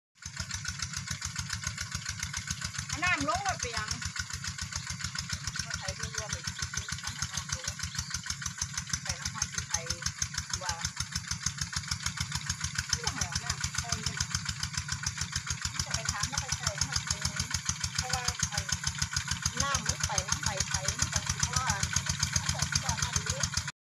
Iron Buffalo at work
Heavy going using an Isaan iron buffalo after rain which has turned this clay into chewing gum consistency.